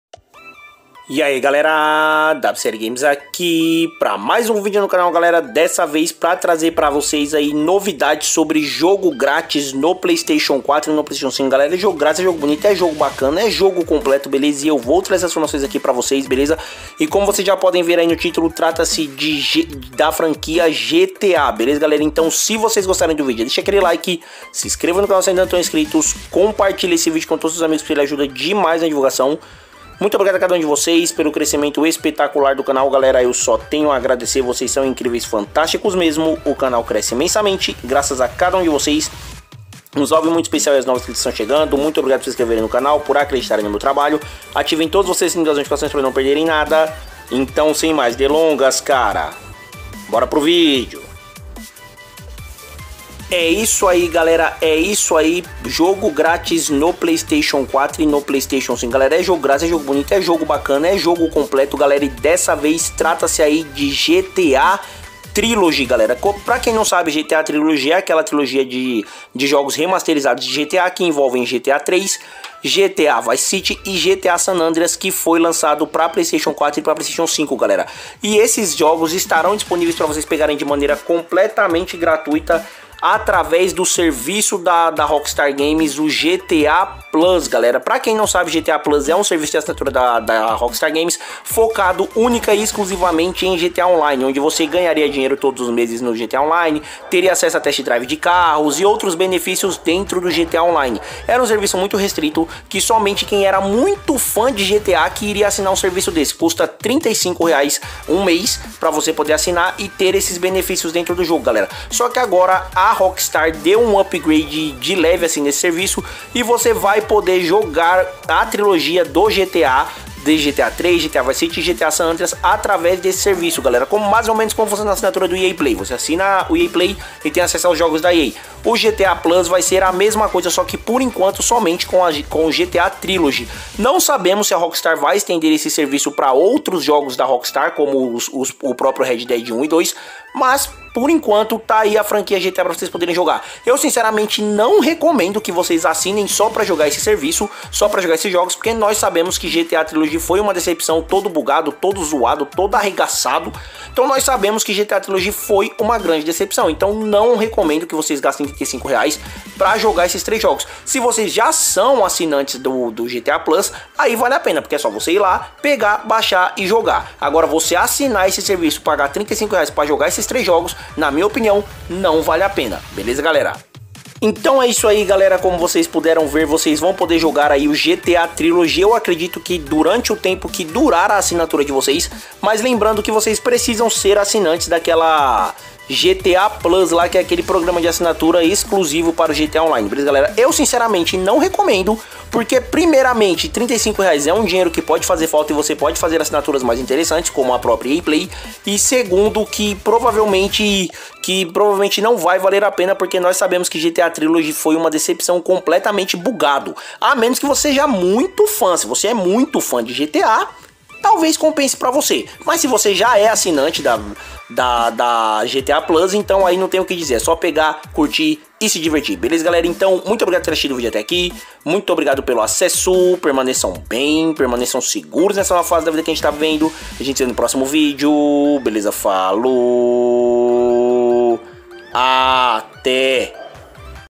jogo gratis no ps4 e ps5 gta gratis no playstation para assinantes rockstar!!!
eae galera wcl games aqui no video de hoje temos mais um jogo gratis no ps4 e ps5 temos dessa vez na plataforma da sony playstation o gta e é possível termos gta gratis no playstation tanto no playstation 4 e no playstation 5 então vamos tornar esse video um viral entre as news sobre games e noticias de jogos gratis para que todos aproveitem